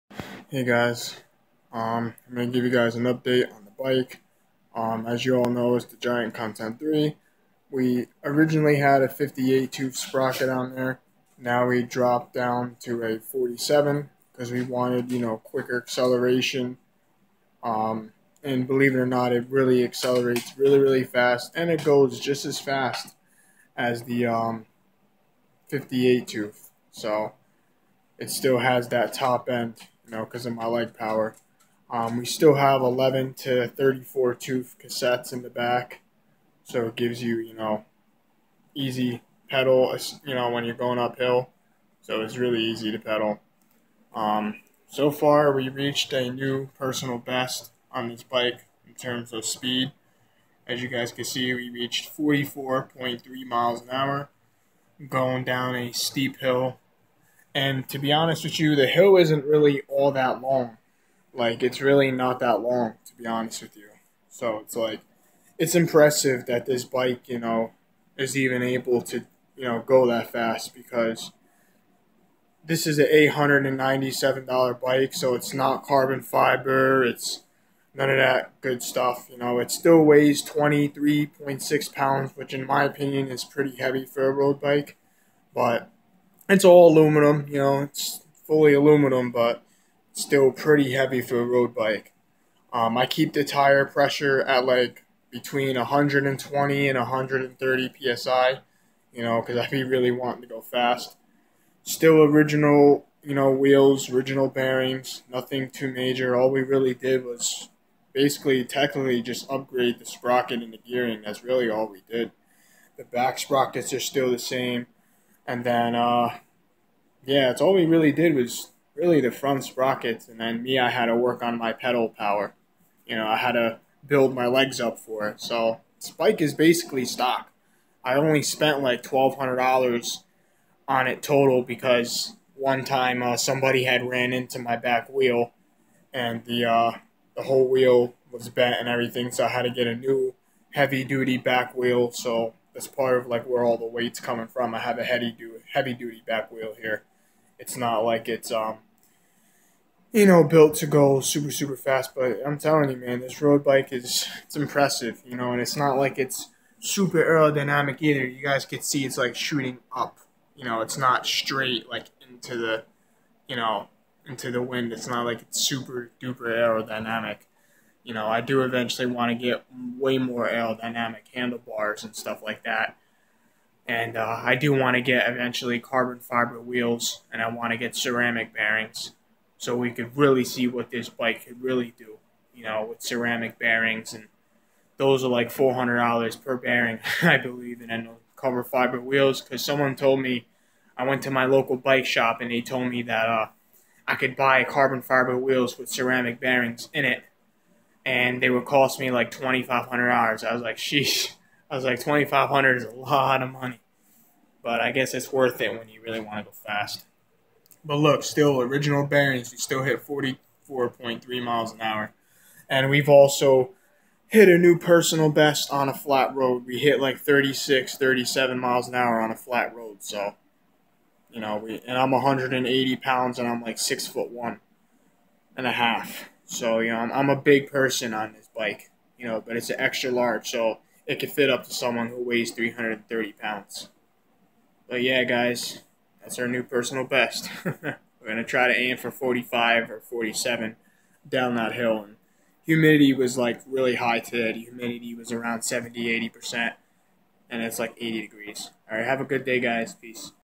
New Personal Best I Went 44.3 mph down a steep hill on My Giant Contend 3!
thiss is my Personal Best on my 2021 Giant Contend 3 Road bike I went 44.3 mph down a steep hill and I went 36-37 mph on a flat Road! I'm 180lbs and 6ft 1! I'm 21 years old!